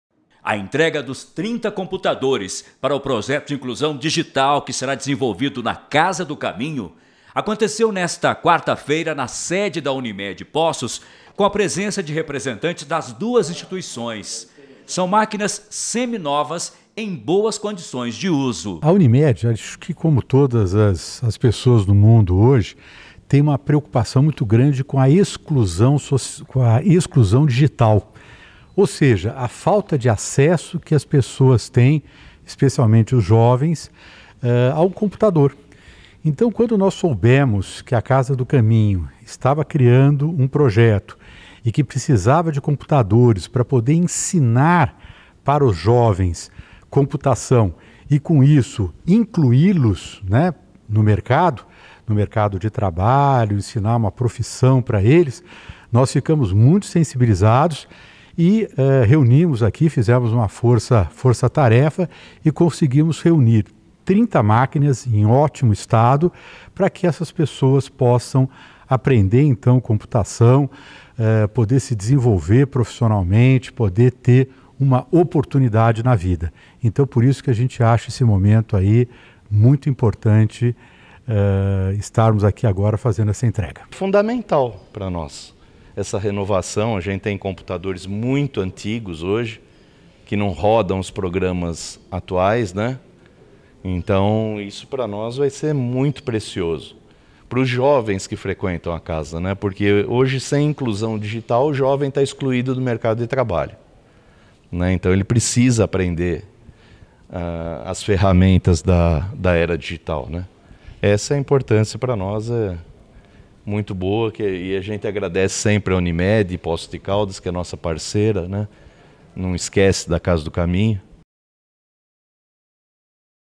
UNIMED FAZ DOAÇÃO CASA DO CAMINHO
A Unimed Poços fez a doação de 30 computadores para projeto de inclusão digital que vai ser desenvolvido na Casa do Caminho.